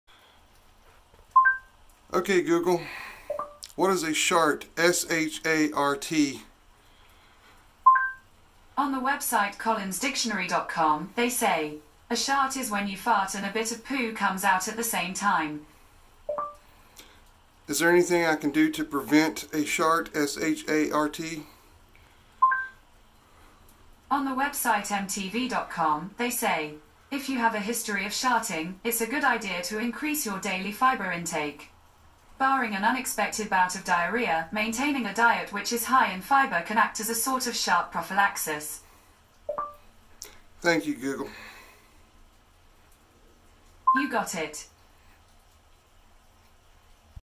I ask Google what is a shart
Having a conversation with Google about sharts